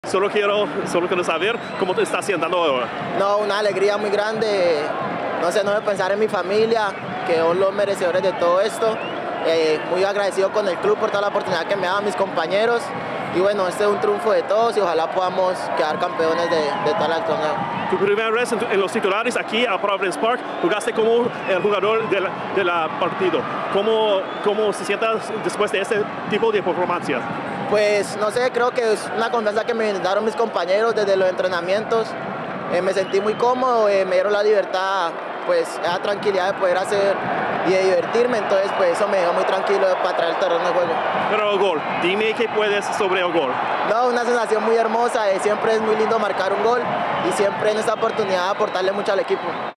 FIELD INTERVIEW | Santiago Moreno talks about his goal and defeating RSL
Timbers midfielder Santiago Moreno talks about his goal and huge performance in Portland's win over RSL in the Audi 2021 MLS Cup Playoffs.